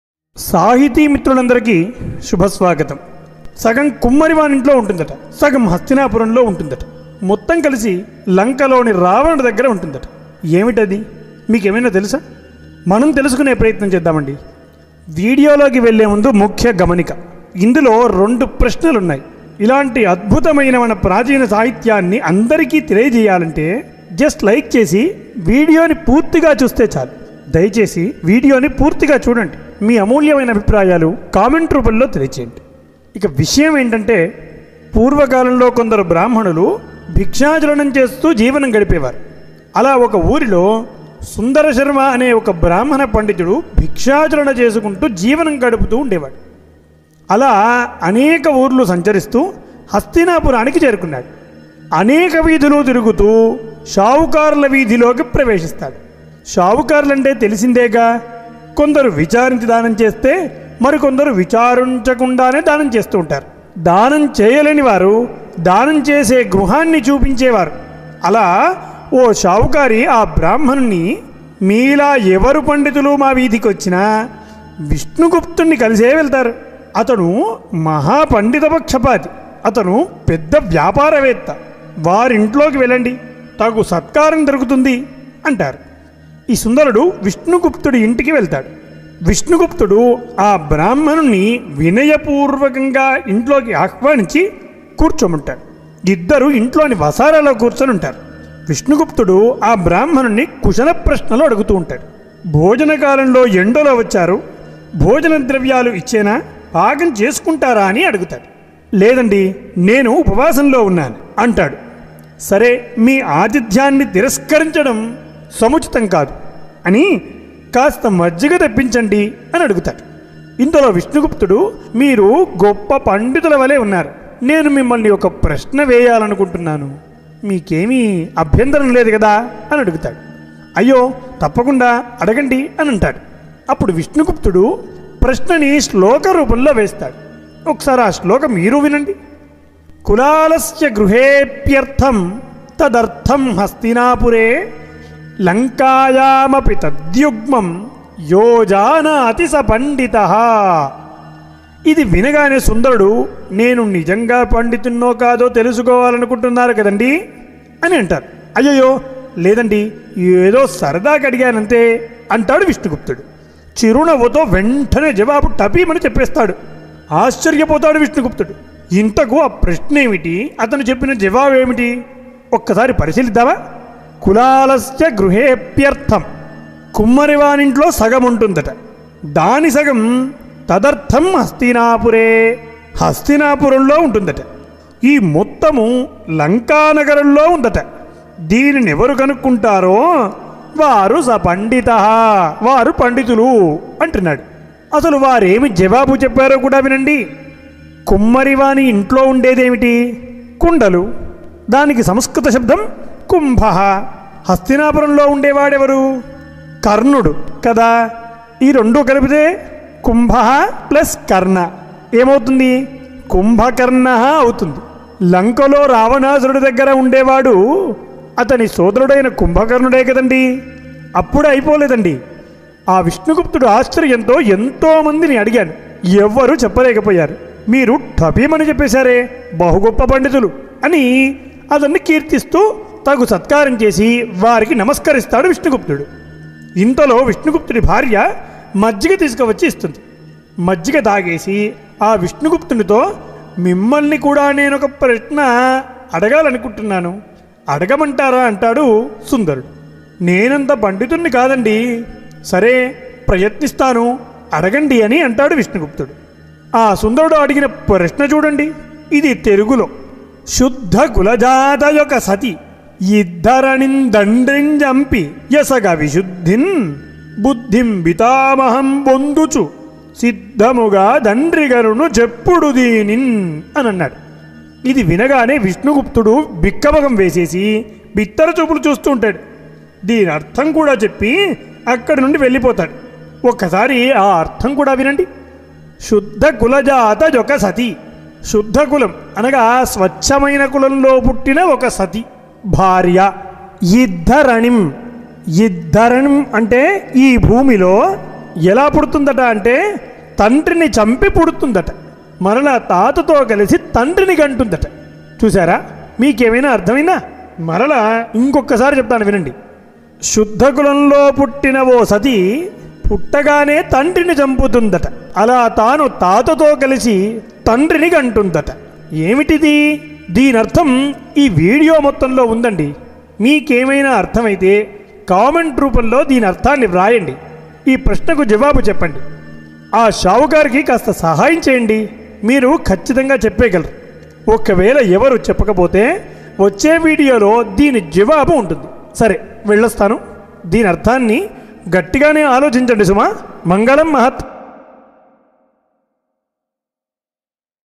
Podupu Kathalu in Telugu | సాహిత్య పొడుపు కథలు | Telugu Stories Morel | New" Kathalu
podupu kathalu in Telugu |
Telugu stories moral

Half the potter is in him! Half is in Hastinapuram! The whole thing is near Ravanasura in Lanka! what is

Do you know anything? Let's try to find out, important note before going into the video, there are two questions in it, just like and watch the full video for your part, to let everyone know about this wonderful ancient literature of ours.

 And the thing is.